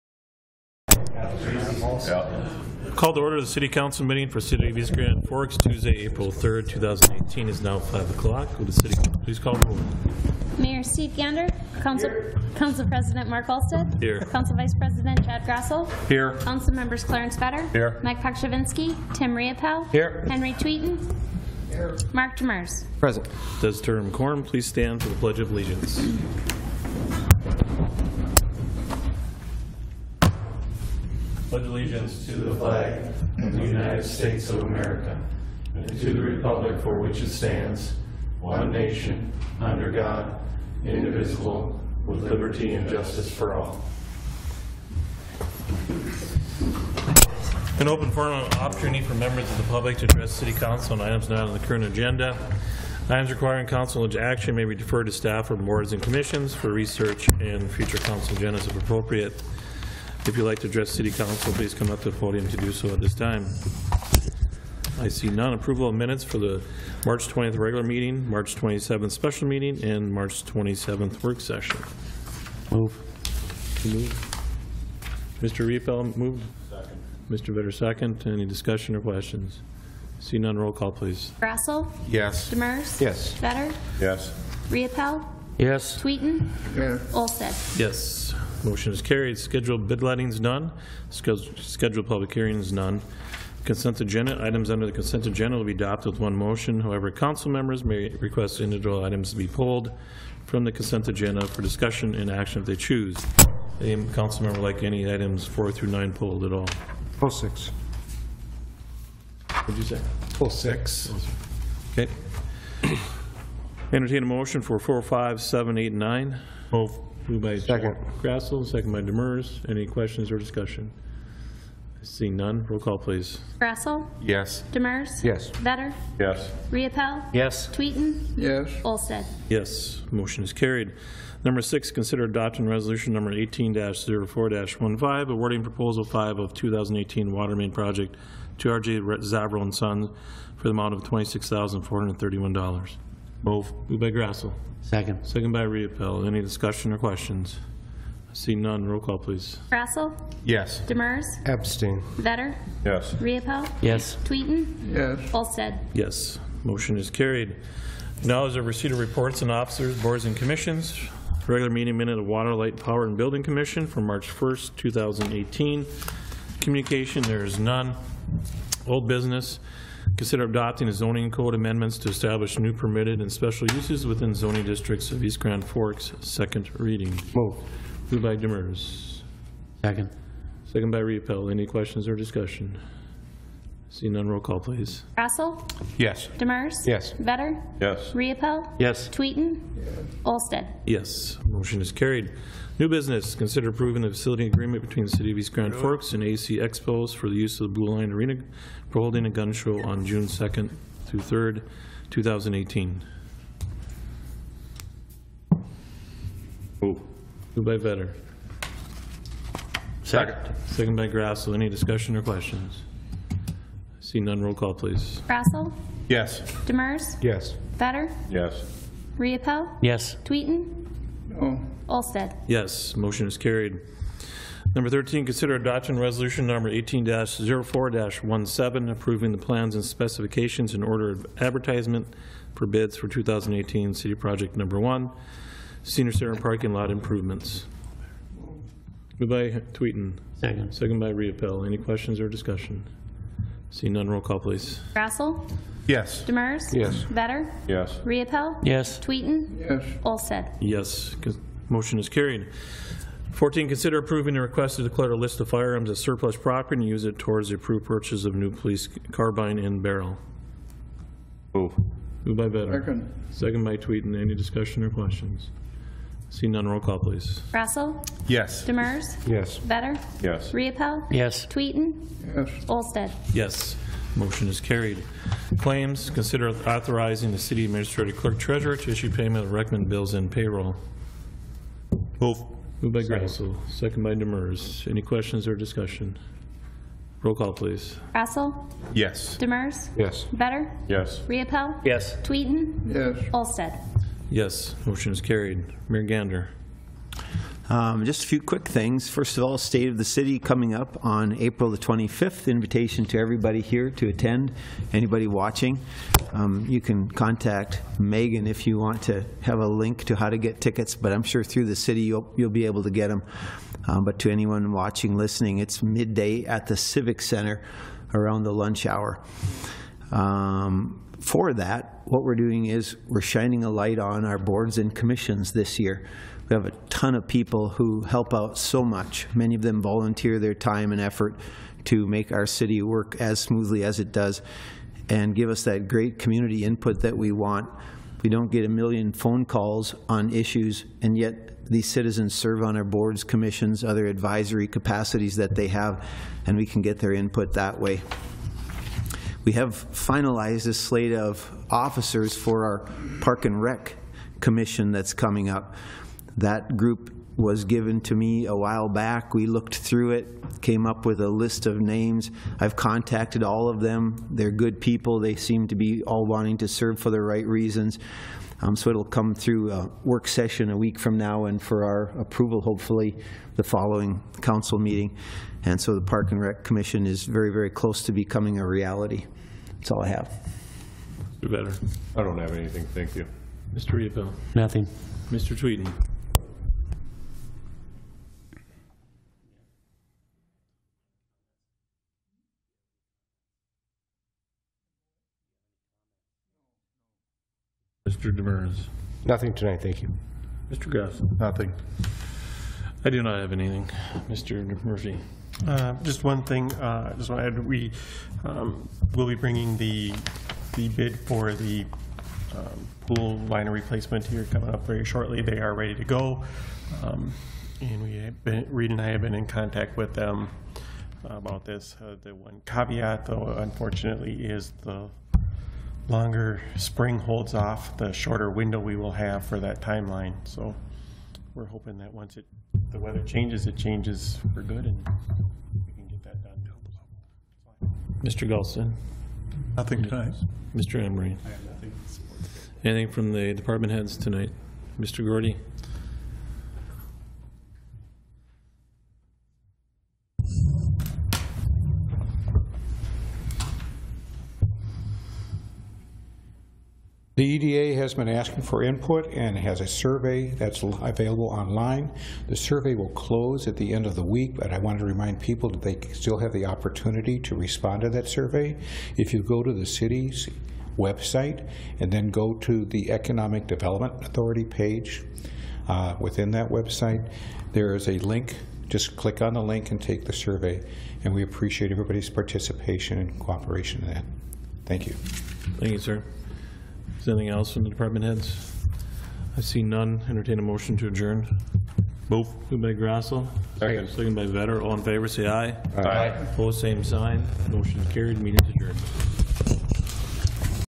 CITY OF EAST GRAND FORKS CITY COUNCIL MEETING TUESDAY, APRIL 3, 2018
AGENDA
OF THE CITY COUNCIL
CITY OF EAST GRAND FORKS
TUESDAY, APRIL 3, 2018 – 5:00 PM
CALL TO ORDER:
CALL OF ROLL:
DETERMINATION OF QUORUM:
PLEDGE OF ALLEGIANCE:
OPEN FORUM:
“An opportunity for members of the public to address the City Council on items not on the current Agenda.
Items requiring Council action maybe deferred to staff or Boards and Commissions for research and
future Council Agendas if appropriate.” If you would like to address the City Council, please come up to
the podium to do so.”
APPROVAL OF MINUTES:
1. Consider approving the minutes of the “Regular Meeting” for the East Grand Forks Minnesota City
Council of March 20, 2018.
2. Consider approving the minutes of the “Special Meeting” for the East Grand Forks Minnesota City
Council of March 27, 2018.
3. Consider approving the minutes of the “Work Session” for the East Grand Forks Minnesota City
SCHEDULED BID LETTINGS: NONE
SCHEDULED PUBLIC HEARINGS: NONE

CONSENT AGENDA:
Items under the “Consent Agenda” will be adopted with one motion; however, council members may
request individual items to be pulled from the consent agenda for discussion and action if they choose.
4. Consider approving the revised farm lease agreement between the City of East Grand Forks and
O’leary, Anderson, and McMahon and will terminate as of December 31, 2020.
5. Consider adopting Resolution No. 18-04-14 authorizing the hiring of the temporary positions for the
2018 summer season.

6. Consider adopting Resolution No. 18-04-15 awarding Proposal 5 of the 2018 Water Main Project to
RJ Zavoral & Sons for the amount of $26,431.
7. Consider adopting Resolution No. 18-04-16 approving the plans and specifications for 2018
Assessment Job No. 1 Street & Pedestrian Improvements and authorizing advertising once final
approval is received by Minnesota Department of Transportation.
8. Consider approving the Special Event Application for the VFW Post 3817 to host a BBQ competition
on April 21, 2018 from 7:00am until 5:30pm in the parking lot area located by the VFW Building.
9. Consider approving a Temporary Liquor License Application for the VFW Post 3817 for a BBQ
competition event taking place on April 21, 2018 in the parking lot area located by the VFW Building.
ACKNOWLEDGE RECEIPT OF REPORTS OF OFFICERS, BOARDS, AND COMMISSIONS:
10. Regular meeting minutes of the Water, Light. Power, and Building Commission for March 1, 2018.
COMMUNICATIONS: NONE
OLD BUSINESS:
11. Consider approving the zoning code amendments to establish new permitted and special uses within
the Zoning Districts of East Grand Forks (2nd Reading).
NEW BUSINESS:
12. Consider approving the facility agreement between the City of East Grand Forks and A.C. Expos for
the use of the Blue Line Arena for holding a gun show on June 2-3, 2018.
13. Consider adopting Resolution No. 18-04-17 approving the plans and specifications and order
advertisement for bids for 2018 City Project No 1 – Senior Center Parking Lot Improvements.
14. Consider approving the request to declare the list of firearms as surplus property and use it towards
the approved purchase of a new police carbine and barrel.
CLAIMS:
15. Consider authorizing the City Administrator/Clerk-Treasurer to issue payment of recommended bills
and payroll.
COUNCIL/STAFF REPORTS:
ADJOURN:
Upcoming Meetings:
Work Session – Tuesday, April 10, 2018 – 5:00 PM – Training Room
Regular Council Meeting – Tuesday, April 17, 2017 – 5:00 PM – Council Chambers
Work Session – Tuesday, April 24, 2018 – 5:00 PM – Training Room
Regular Council Meeting – Tuesday, May 1, 2018 – 5:00 PM – Council Chambers